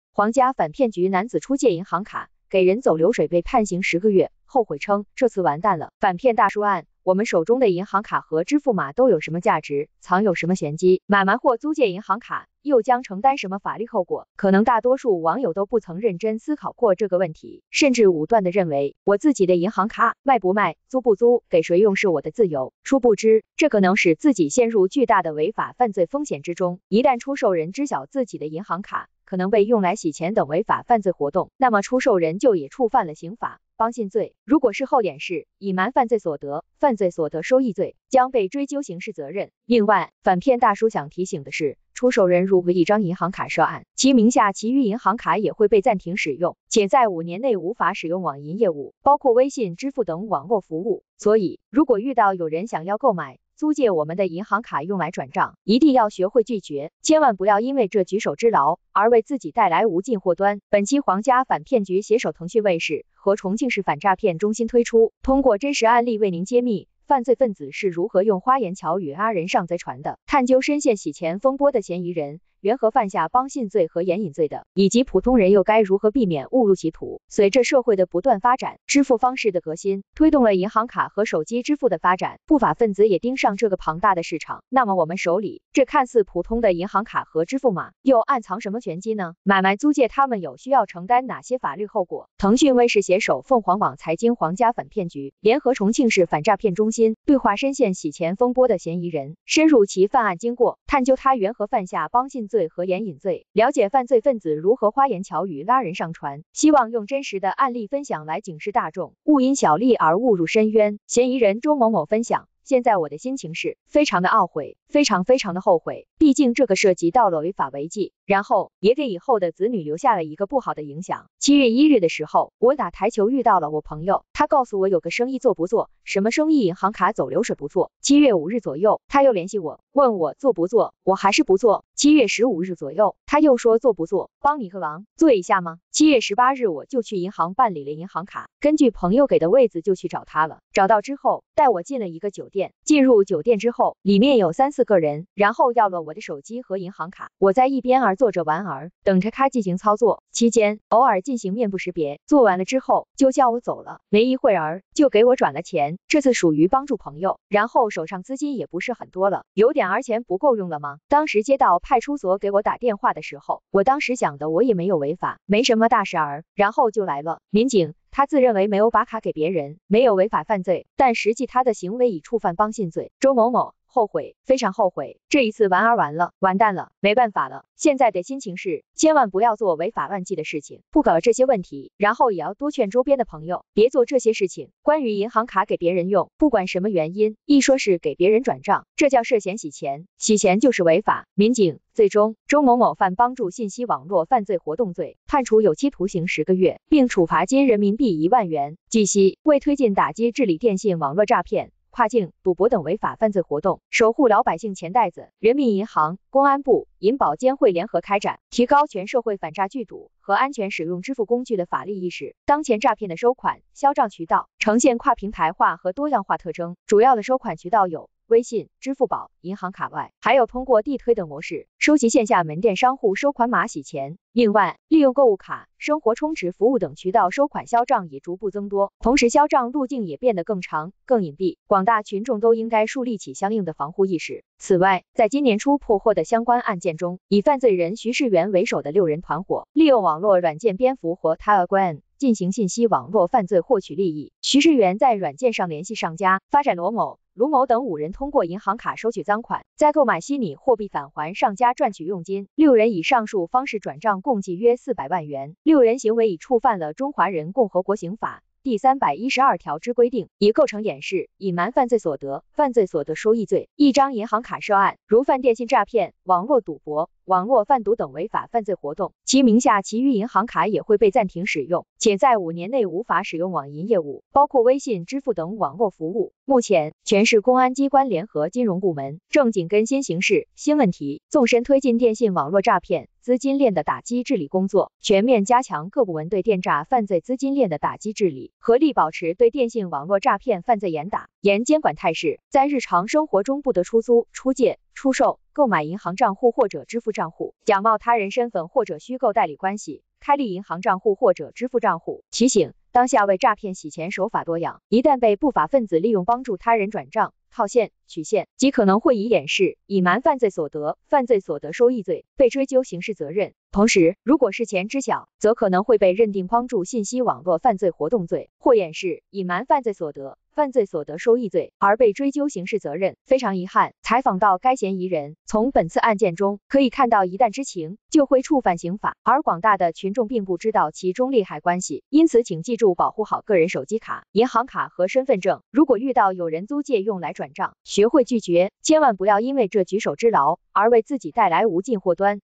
凰家反骗局|男子出借银行卡给人走流水被判刑10个月 后悔称：这次完蛋了！
后悔 非常后悔 这一次玩儿完了 完蛋了 没办法了 现在的心情是 千万不要做违法乱纪的事情 不搞这些问题 然后也要多劝周边的朋友 别做这些事情 关于银行卡给别人用 不管什么原因 一说是给别人转账 这叫涉嫌洗钱 洗钱就是违法 民警 手法多样 一旦被不法分子利用帮助他人转账、套现、取现 极可能会以掩饰、隐瞒犯罪所得、犯罪所得收益罪 被追究刑事责任 同时 如果事前知晓 则可能会被认定帮助信息网络犯罪活动罪 或掩饰、隐瞒犯罪所得、犯罪所得收益罪 而被追究刑事责任 非常遗憾 采访到该嫌疑人 从本次案件中 可以看到一旦知情 就会触犯刑法 而广大的群众并不知道其中厉害关系 因此请记住保护好个人手机卡 银行卡和身份证 如果遇到有人租借用来转账 学会拒绝 千万不要因为这 微信、支付宝、银行卡外 还有通过地推等模式 收集线下门店商户收款码洗钱 另外 利用购物卡、生活充值服务等渠道收款销账也逐步增多 同时销账路径也变得更长、更隐蔽 广大群众都应该树立起相应的防护意识 此外 在今年初破获的相关案件中 以犯罪人徐世元为首的6人团伙 利用网络软件蝙蝠和Telegram 进行信息网络犯罪获取利益 徐世元在软件上联系上家 发展罗某、卢某等5人通过银行卡收取赃款 再购买虚拟货币返还上家赚取佣金 6人以上述方式转账共计约400万元 6人行为已触犯了《中华人共和国刑法》第三百一十二条之规定 已构成掩饰、隐瞒犯罪所得、犯罪所得收益罪 一张银行卡涉案 如犯电信诈骗、网络赌博、网络贩毒等违法犯罪活动 其名下其余银行卡也会被暂停使用 且在5年内无法使用网银业务 包括微信支付等网络服务 目前 全市公安机关联合金融部门 正紧跟新形势 新问题 纵深推进电信网络诈骗、资金链的打击治理工作 全面加强各部门对电诈犯罪资金链的打击治理 合力保持对电信网络诈骗犯罪严打 严监管态势 在日常生活中不得出租、出借、出售、购买银行账户或者支付账户 假冒他人身份或者虚构代理关系开立银行账户或者支付账户 提醒 帮信罪 如果事后掩饰、隐瞒犯罪所得、犯罪所得收益罪 将被追究刑事责任 另外 反骗大叔想提醒的是 犯罪分子是如何用花言巧语拉人上贼船的 探究深陷洗钱风波的嫌疑人缘何犯下帮信罪和掩隐罪的 以及普通人又该如何避免误入歧途 随着社会的不断发展 支付方式的革新 推动了银行卡和手机支付的发展 不法分子也盯上这个庞大的市场 那么我们手里 这看似普通的银行卡和支付码 又暗藏什么玄机呢 买卖租借他们有需要承担哪些法律后果 腾讯卫士携手凤凰网财经《凰家反骗局》 联合重庆市反诈骗中心 对话深陷洗钱风波的嫌疑人 深入其犯案经过 探究他缘何犯下帮信罪和掩隐罪 了解犯罪分子如何花言巧语拉人上船 希望用真实的案例分享来警示大众 勿因小利而误入深渊 嫌疑人周某某分享 出售人如果一张银行卡涉案 其名下其余银行卡也会被暂停使用 且在5年内无法使用网银业务 包括微信支付等网络服务 所以 如果遇到有人想要购买、租借我们的银行卡用来转账 一定要学会拒绝 千万不要因为这 帮你个忙 做一下嘛 7月18日我就去银行办理了银行卡 根据朋友给的位子就去找他了 找到之后 带我进了一个酒店 进入酒店之后 里面有三四个人 然后要了我的手机和银行卡 我在一边儿坐着玩儿 等着他进行操作 期间 偶尔进行面部识别 做完了之后 就叫我走了 没一会儿 就给我转了钱 这次属于帮助朋友 然后手上资金也不是很多了 有点儿钱不够用了嘛 当时接到派出所给我打电话的时候 我当时想的 凰家反骗局|男子出借银行卡给人走流水被判刑10个月 后悔称